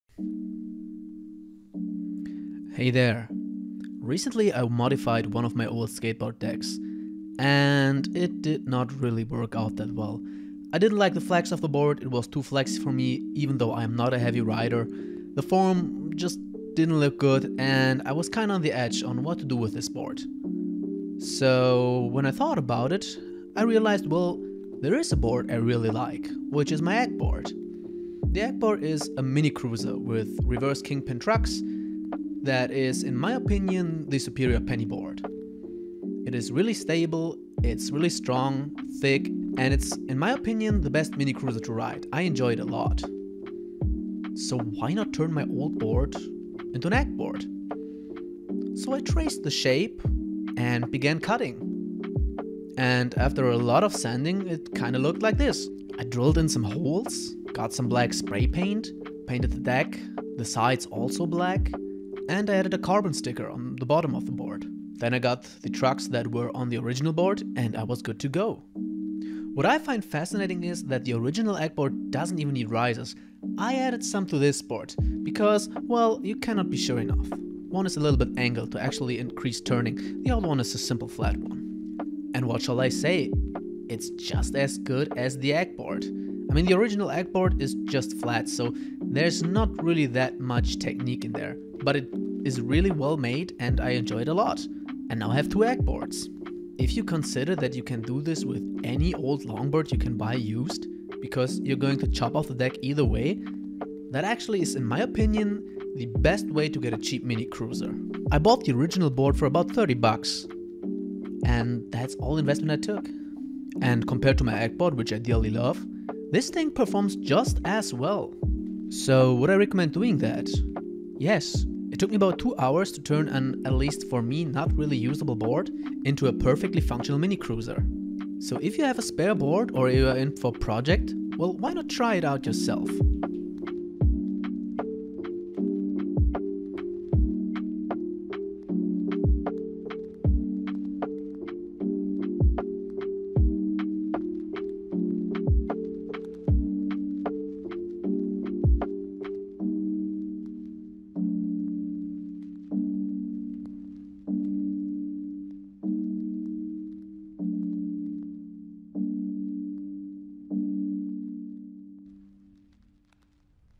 Build your own Mini Cruiser - DIY Mini Longboard Skateboard
In this Video I turn my old Longboard Cruiser into an Eggboard.